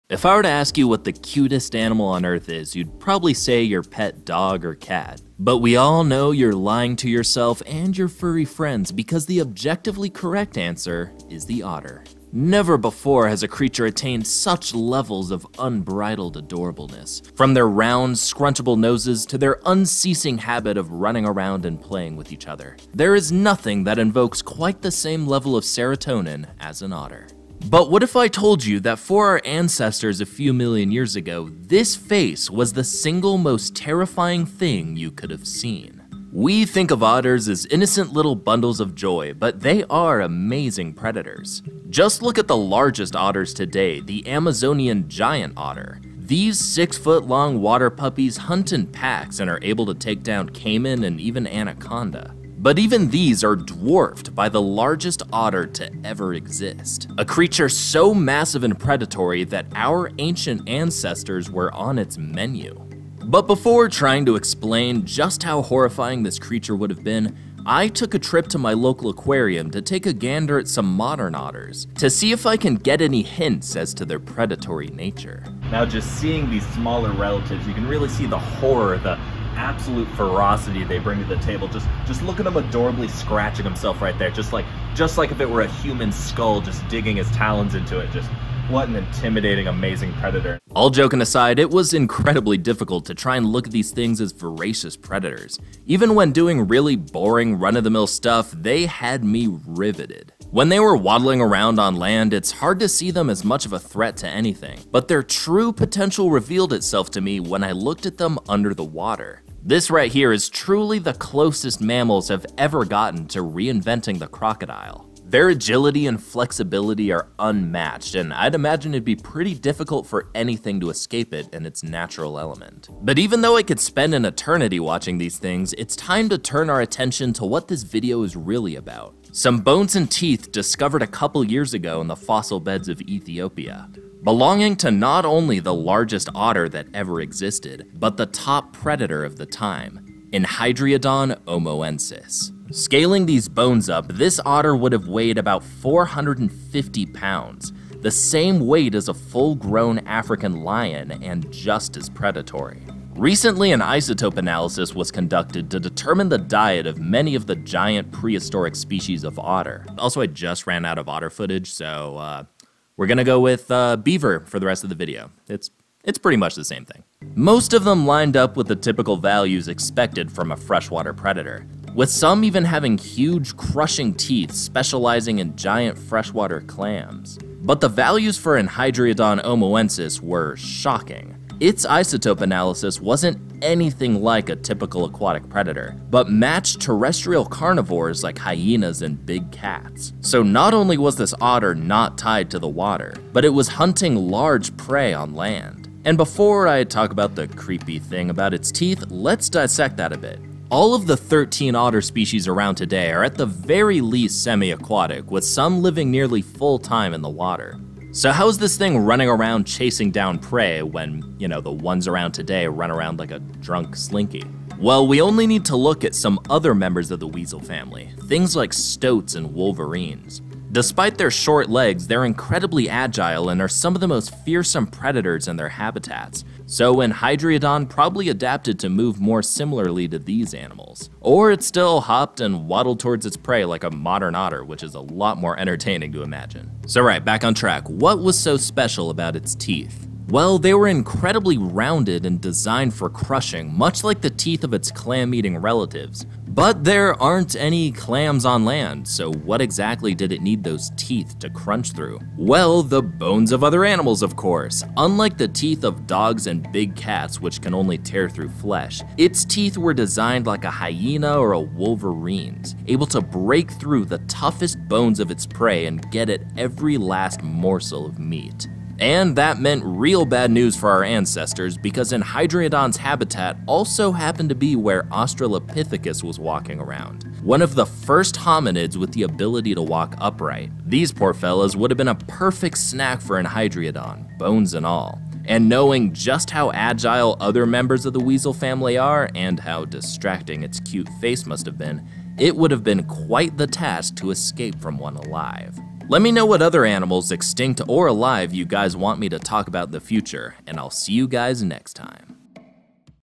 The Prehistoric Otter that ATE our Ancestors...
Enhydriodon omoensis was the biggest otter that ever lived andate our prehistoric relative Australopithecus. Learn all about it here!